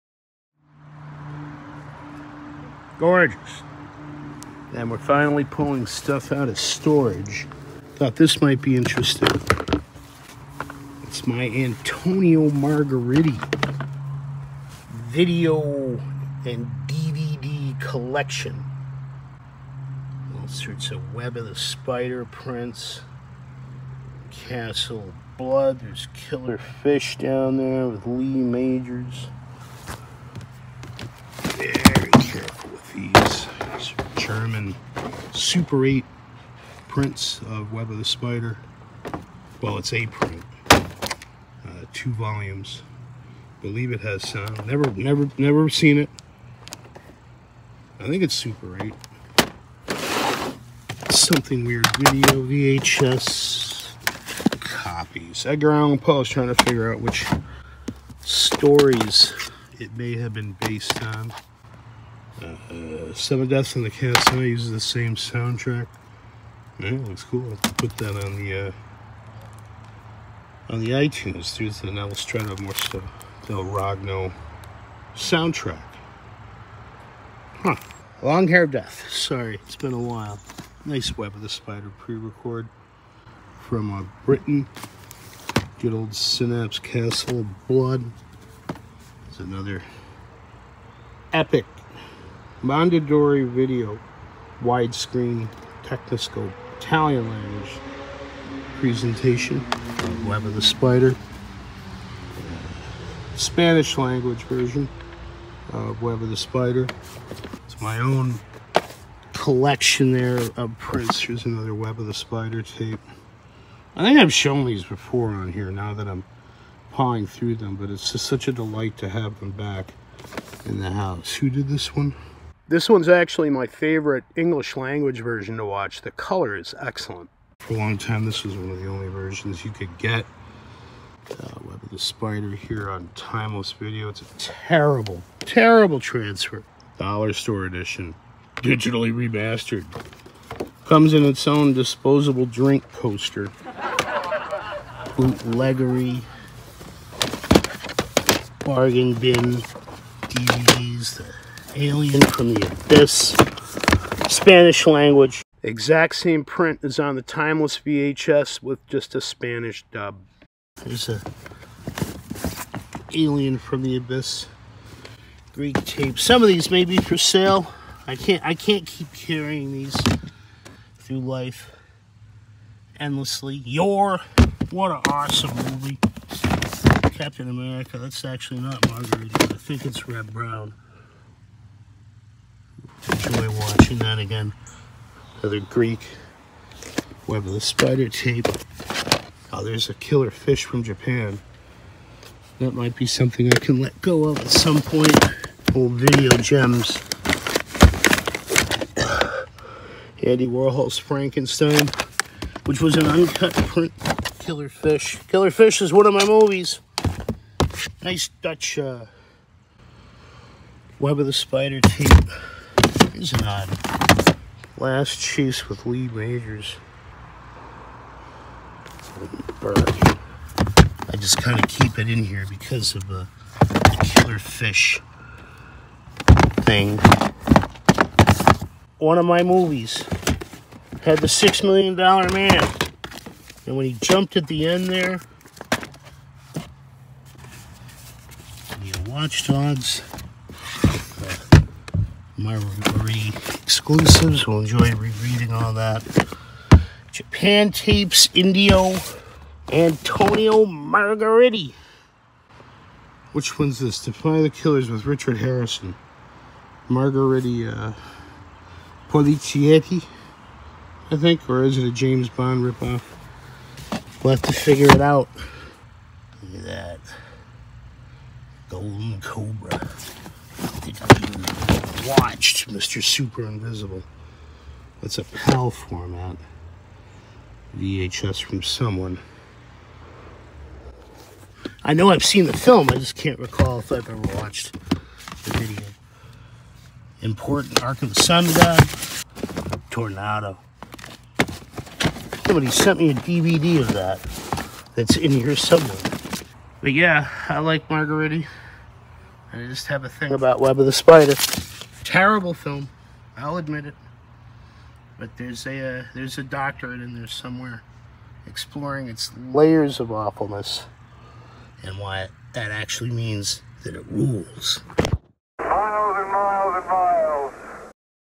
Antonio Margheriti VHS/DVD/Super 8 Collection, including Web of the Spider & Killer Fish Japan VHS
Have shown the Web of the Spider tapes before but was so glad to see them again the mind blanked. Margheriti spaghetti western & Peplum thrillers kept with those genre collections, will get to them soon. And cheers to Tim Lucas for copies of the Riz Ortolani soundtracks, knew they came from somewhere special.

Wish there'd been time to pull out each tape individually, had to skip a lot of stuff that might have been more interesting than the Web tapes again. Will take a closer look once more gets emptied from storage - There's crates of movies to sort through & dispensing of them one of my summer jobs.

Nothing is for sale yet, and I have no such equipment to make copies of anything, sorry. But if you simply must have that Japan "Killer Fish" tape I'll take $150 (US) for it, as-is, plus whatever actual postage would be to your global location. Serious inquiries only please.